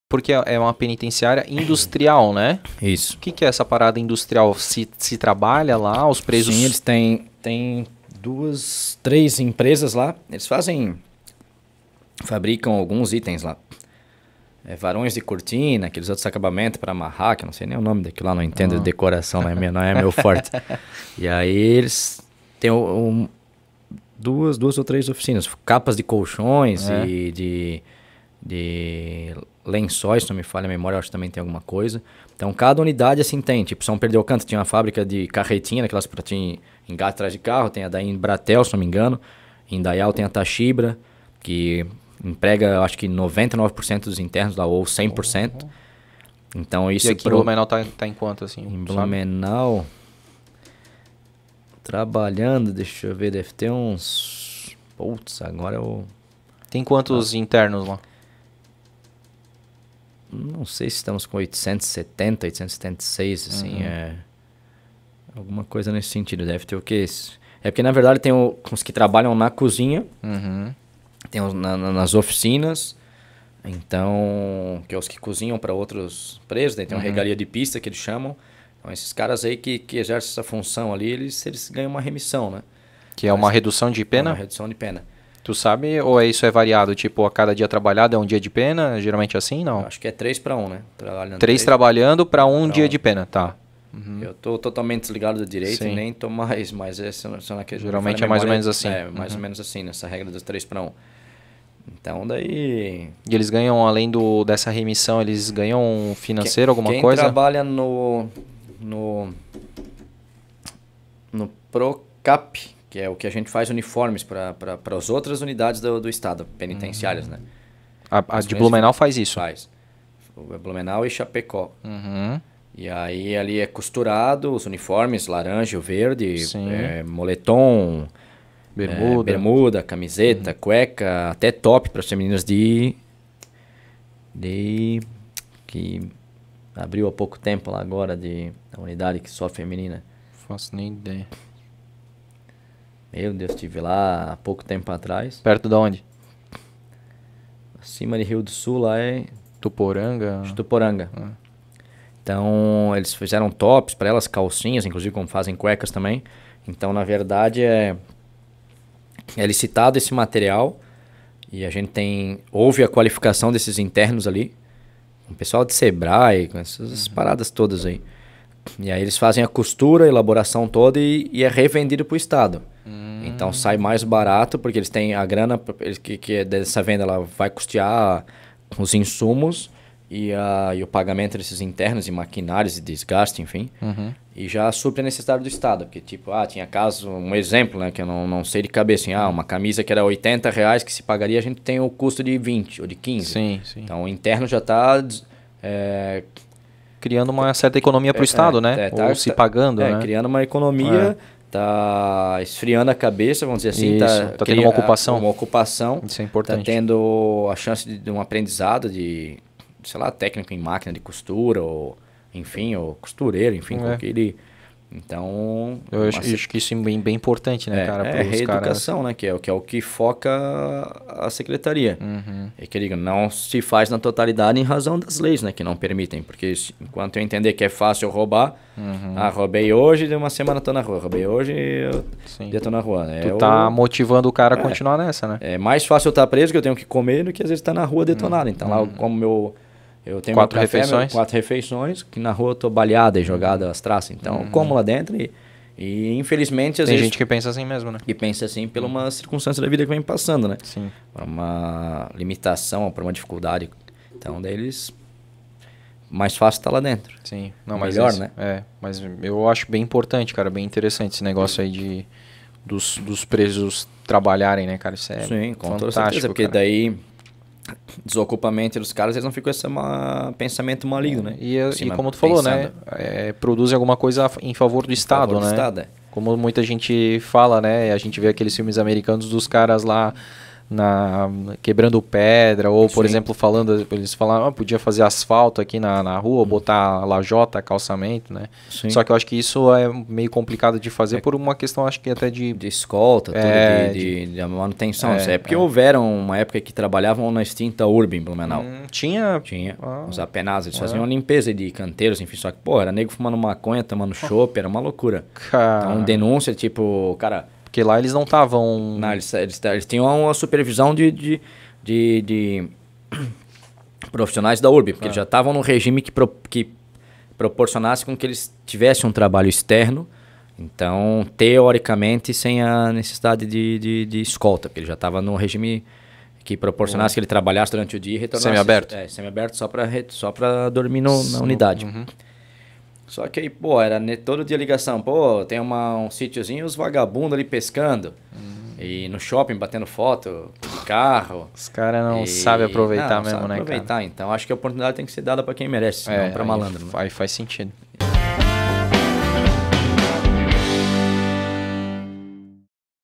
COMO FUNCIONA UMA PENITENCIÁRIA INDUSTRIAL?
Convidado:  André Piazera (policial penal)
Host: André Cantoni

Siga a gente no instagram: @blumencast